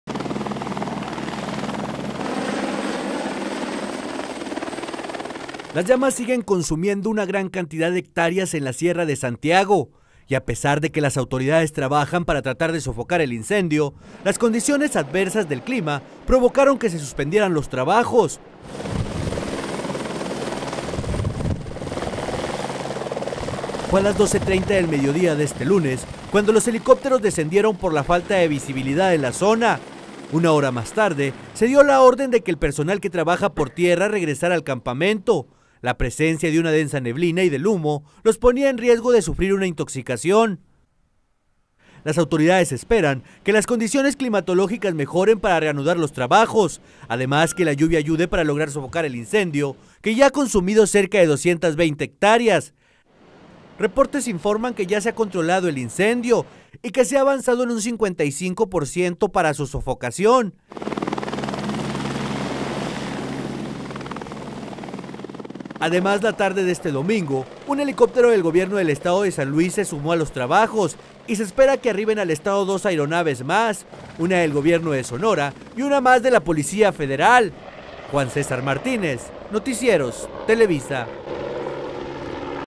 Las Noticias - Suspenden labores en Sierra de Santiago
Las condiciones adversas del clima provocaron que se suspendieran las labores para sofocar el incendio de la Sierra de Santiago, un helicóptero del Estado de San Luis se sumó a los trabajos de sofocación.